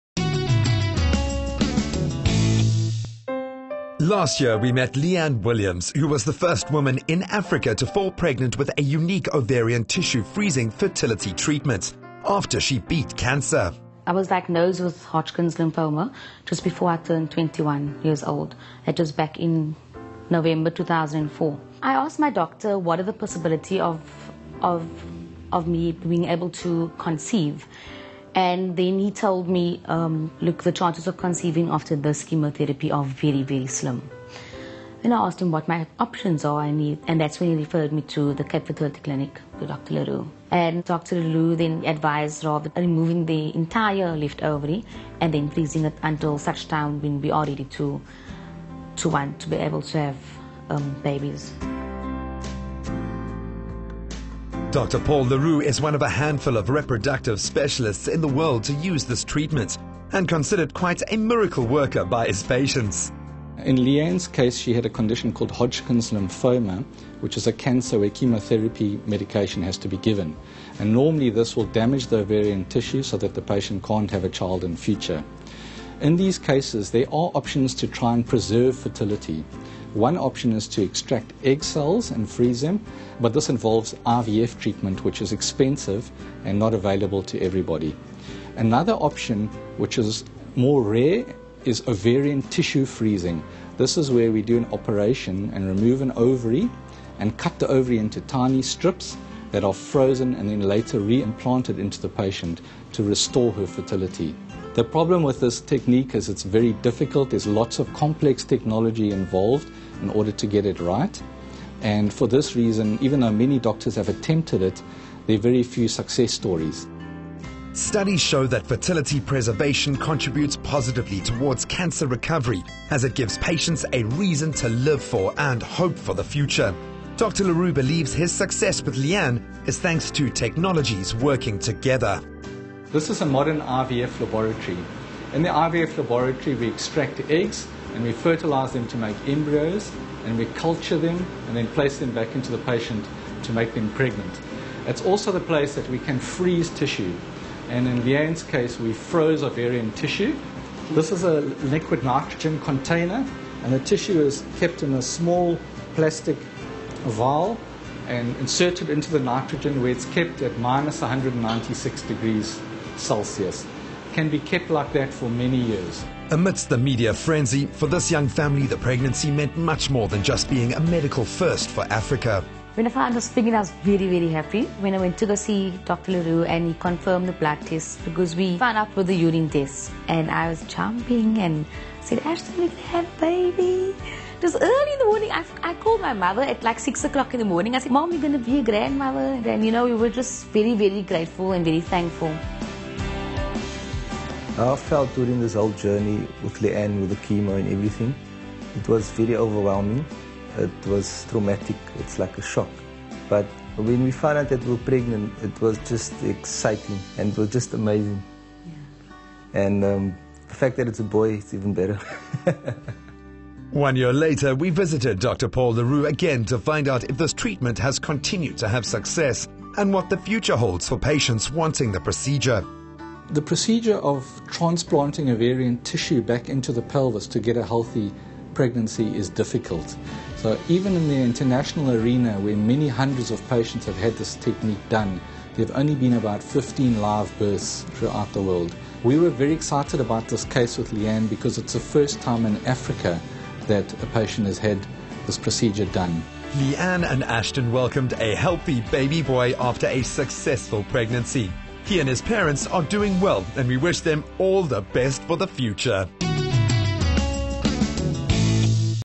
Ovarian Freeze Revisit (13 June 2014)
At 21 years old Lee-Anne Williams was diagnosed with Hodgkin's lymphoma, a cancer affecting the white blood cells.

Although treatable with chemotherapy, the intense treatment would almost certainly destroy her ovaries, and with that her chances of pregnancy. Luckily Dr Paul le Roux from the cape fertility clinic knew exactly what to do to preserve Lee Ann's chances of starting a family.